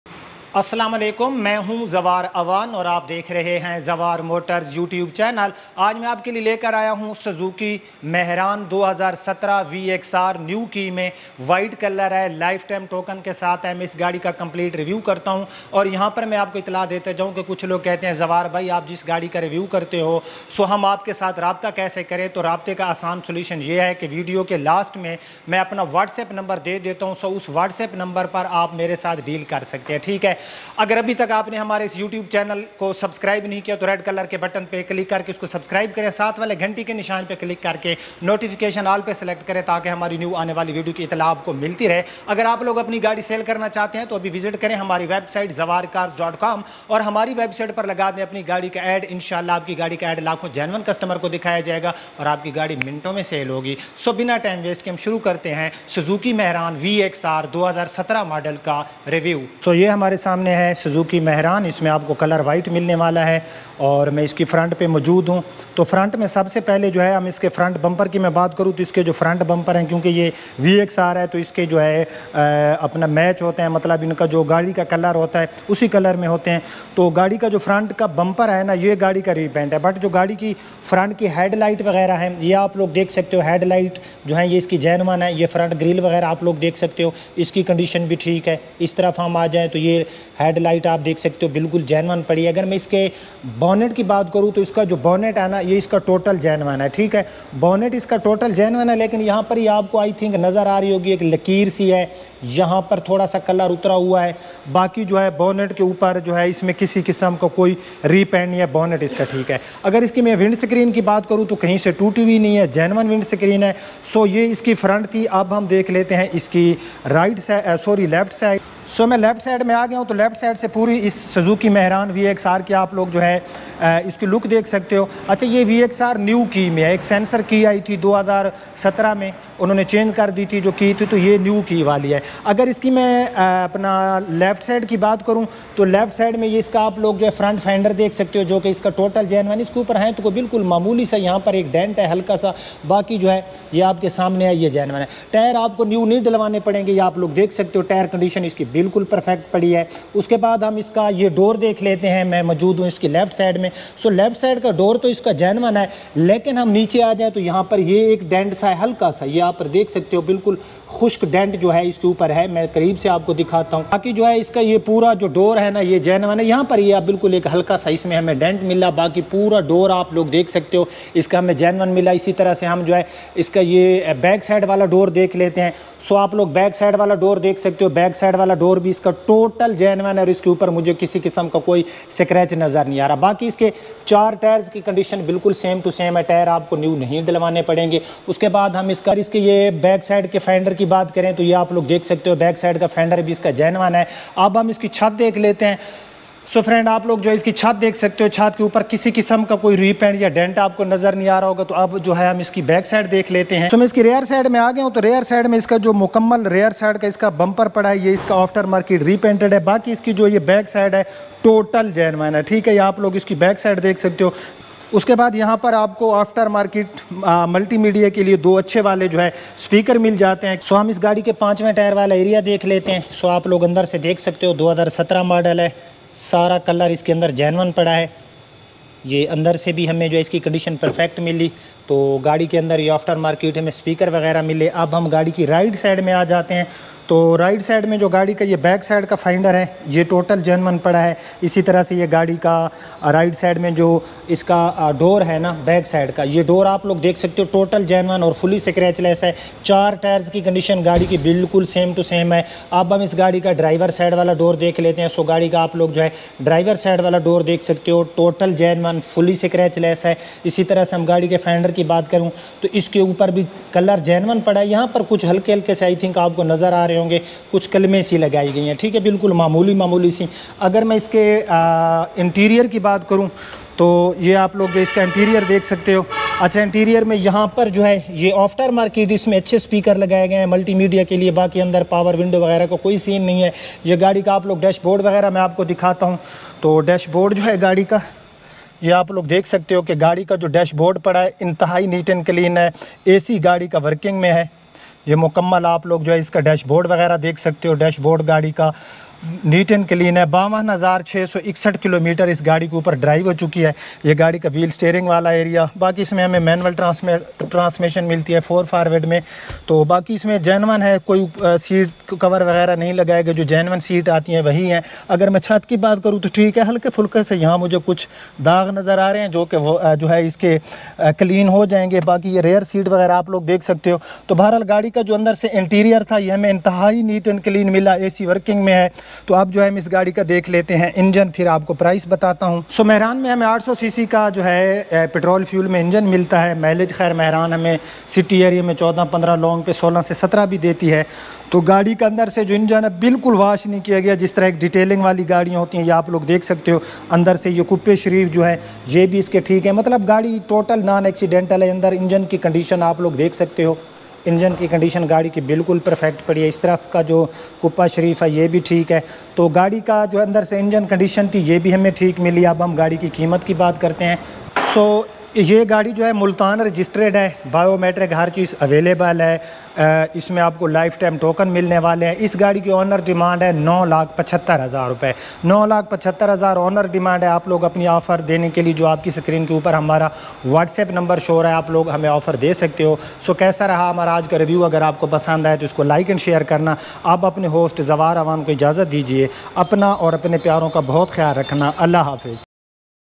Suzuki Mehran VXR 2017 Outclass Condition Price Full Review For Sale In Zawar Motors
Suzuki Mehran VXR Model 2017 Life Token Multan Registered Bumper Repaint Remaining Genuine New Tires Multimedia System Install Mechanical No Work
______________/LINKS\_____________________________